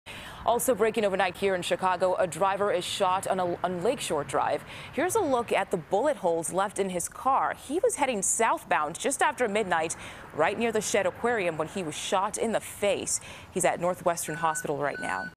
Driver Shot In Face On Lakeshore Drive Near Shedd Aquarium
A driver was shot on Lake Shore Drive in Chicago early Sunday morning.